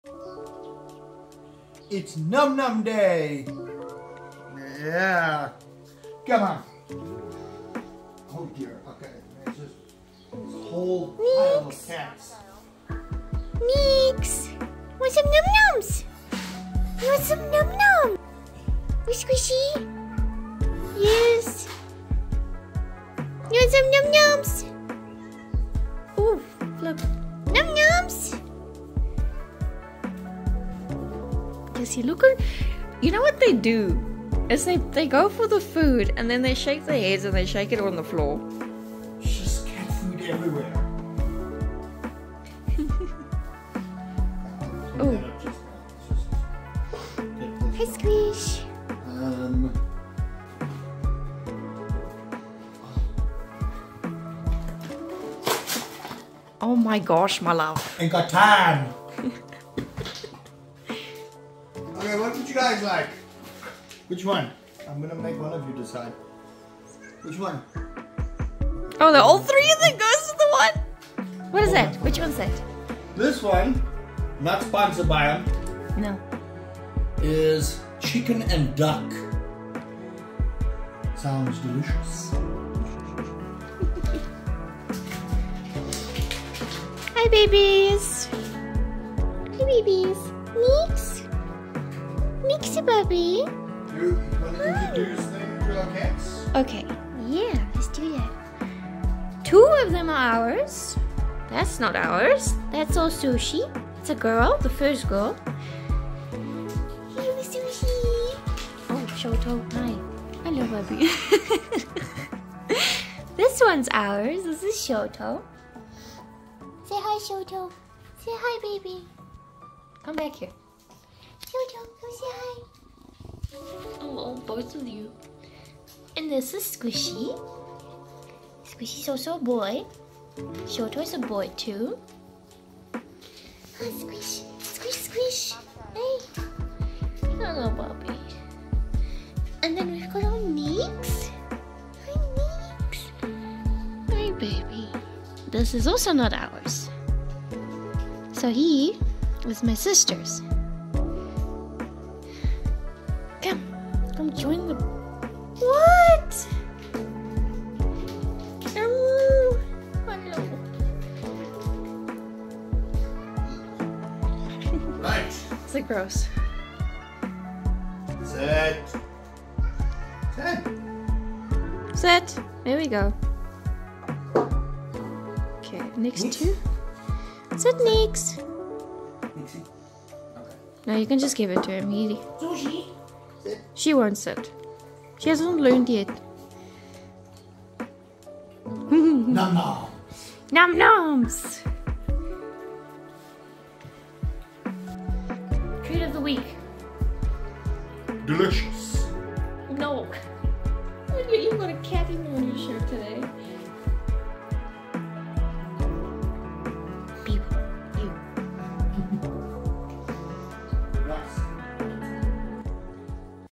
Its Num Num Day! | Every Friday with the cats
Join us as we feed our fur babies their weekly friday wet food called "Num Num Day"

They absolutely LOVE Pampers Gravy range and we love making them choose the flavour of the day!